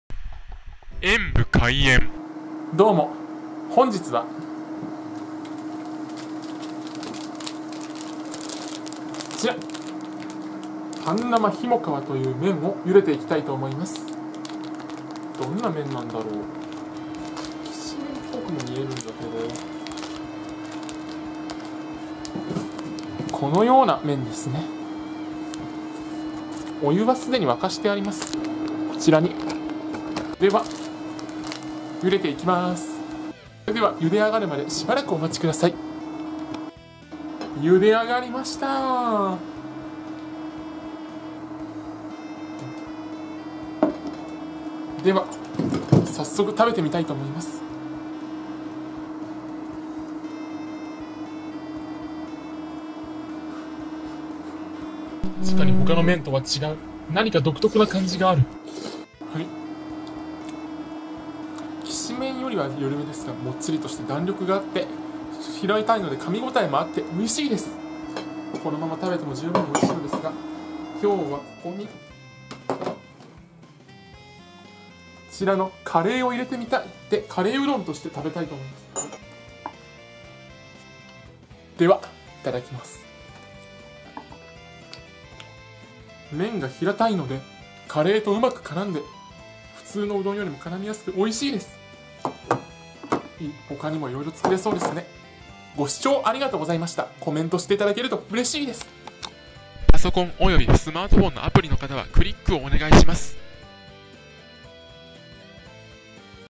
栃木名産 半生ひもかわを食べてみた こんな麺初めて！！
栃木県の名産品半生ひもかわを食べてみました。
饂飩ともきし麺とも違う初めて食べる麺です！！